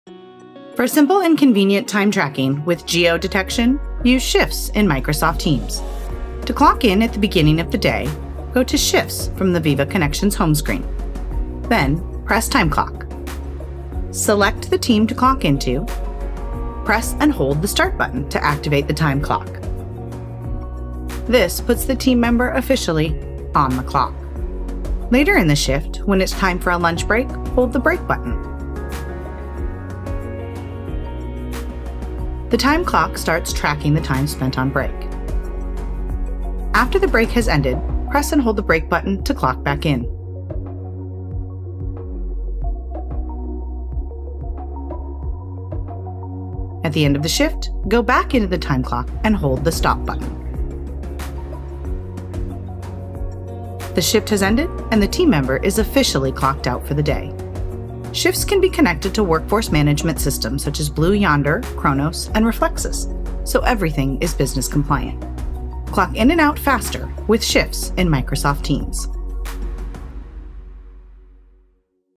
Clocking in with Shifts in Microsoft Teams
See how Shifts in Microsoft Teams provides frontline workers with simple and convenient time tracking.